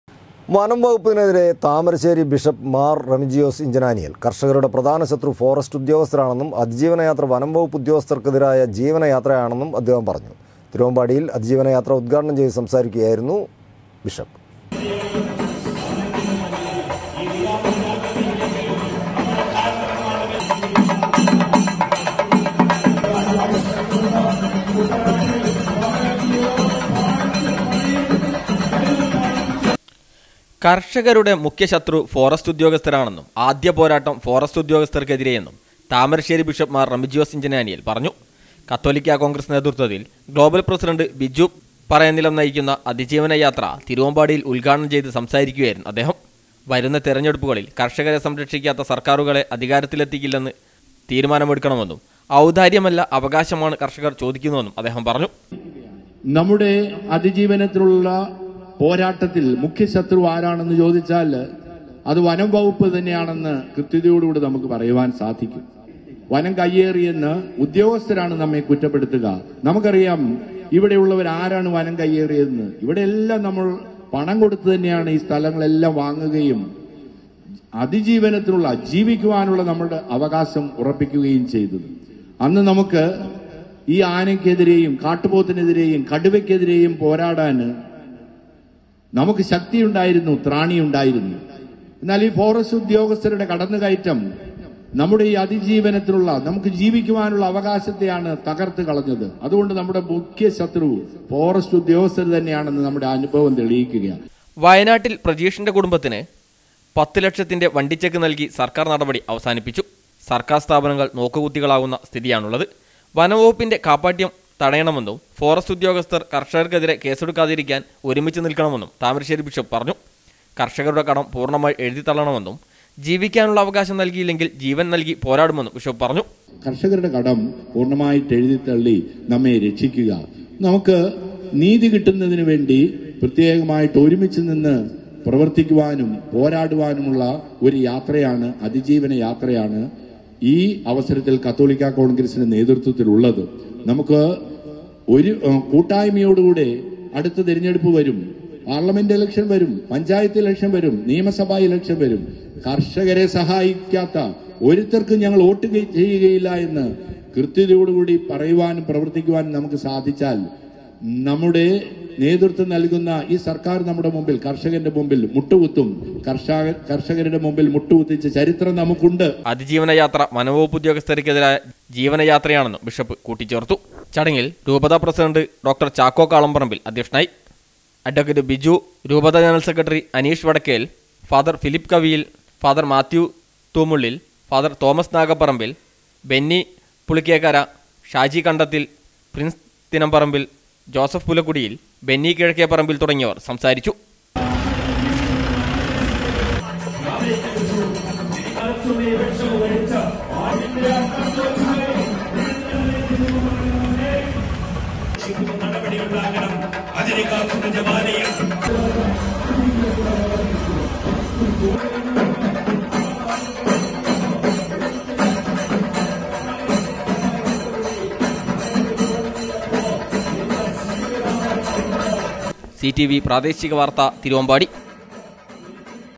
വനംവകുപ്പിനെതിരെ താമരശ്ശേരി ബിഷപ്പ് മാർ റമീജിയോസ് ഇഞ്ചനാനിയിൽ.
വനംവകുപ്പിനെതിരെ താമരശ്ശേരി ബിഷപ്പ് മാർ റമീജിയോസ് ഇഞ്ചനാനിയിൽ. കർഷകരുടെ പ്രധാന ശത്രു ഫോറസ്റ്റ് ഉദ്യോഗസ്ഥരാണന്നും അതിജീവന യാത്ര വനം വകുപ്പ് ഉദ്യോഗസ്ഥർക്കെതിരായ ജീവന യാത്രയാണന്നും അദ്ധേഹം പറഞ്ഞു